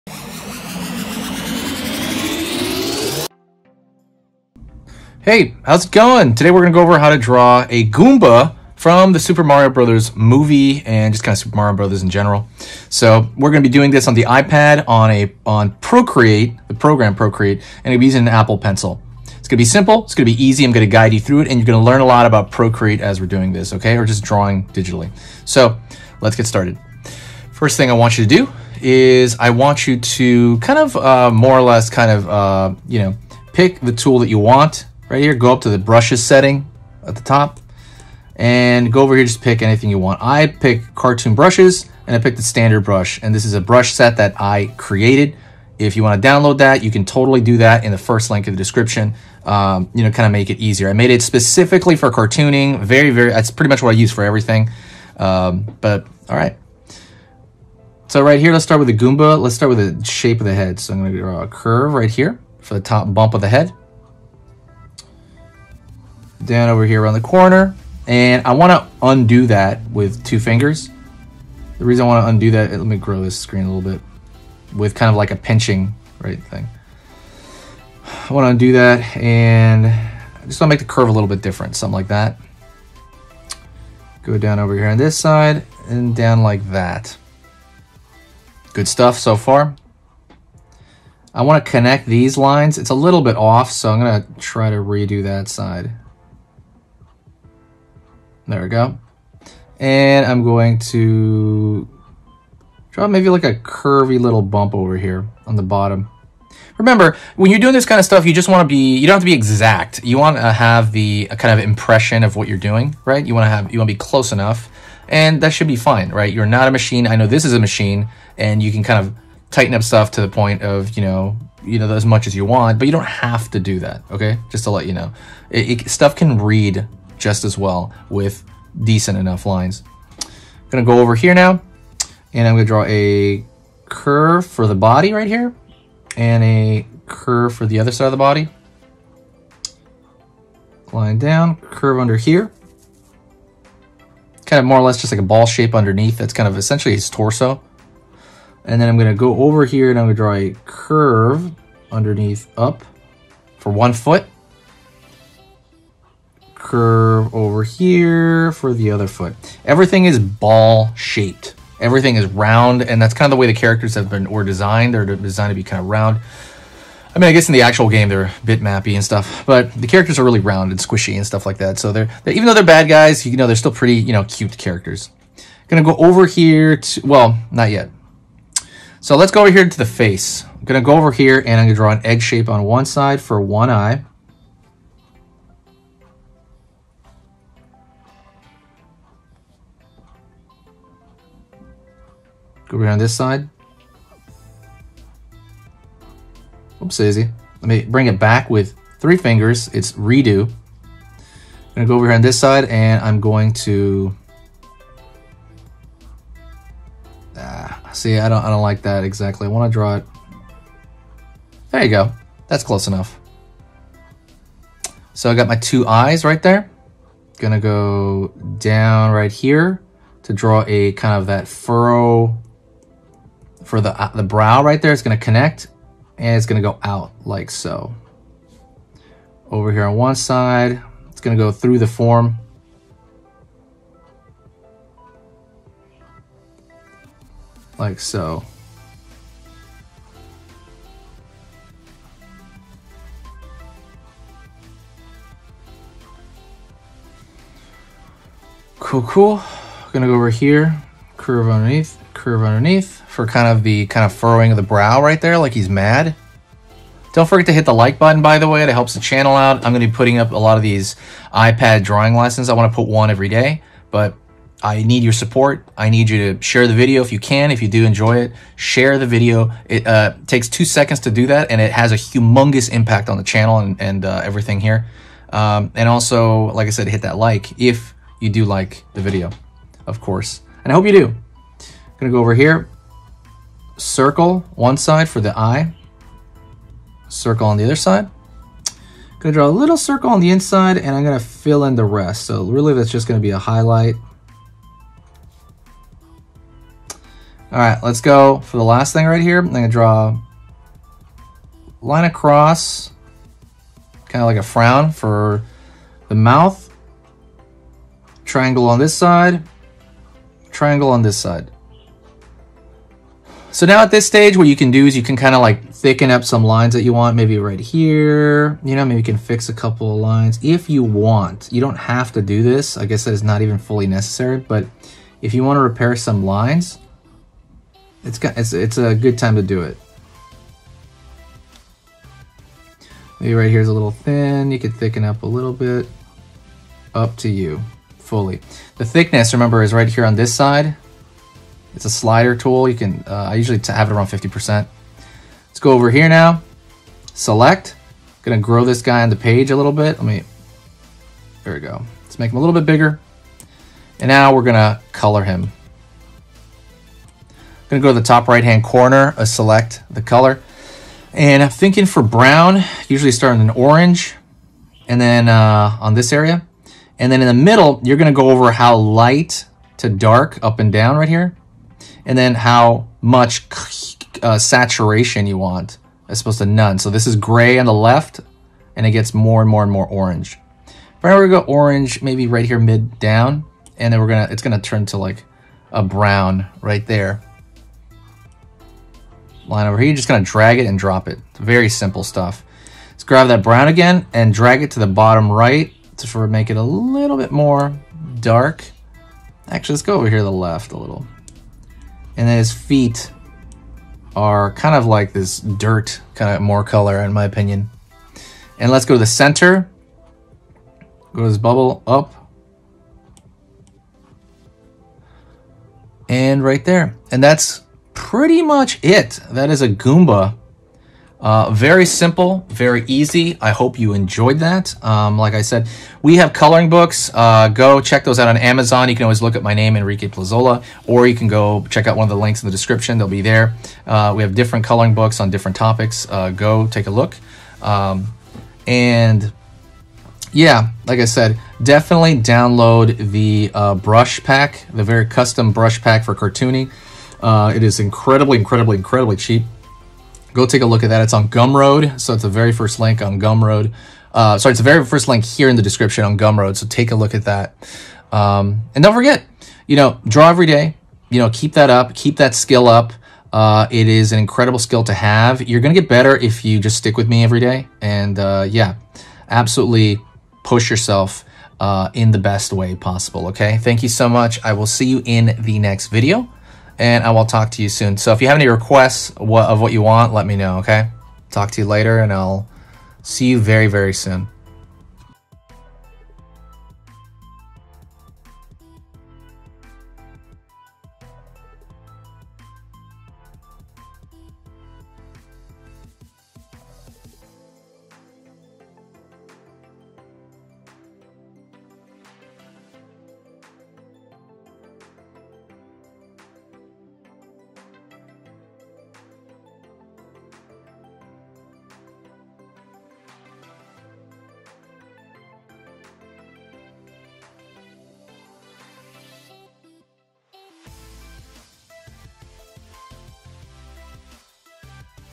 EASY How to Draw MARIO MOVIE - GOOMBA | PROCREATE DRAWING LESSON
Learn to Draw Among Us on Procreate

PICK UP A COLORING BOOK Here:

shorturl.at/mxEY7

Howdy! This is a drawing lesson done on the iPad. We will be using Procreate. We are going to go through this step by step for beginners. i hope you have an amazing day. This is going to be a lesson for anyone. I loved the Mario movie. If you are a Nintendo fan, you'll enjoy this. Please hit the share button.

Cool Links ___________________________________________________

Easy Tips on How to Draw DINOSAURS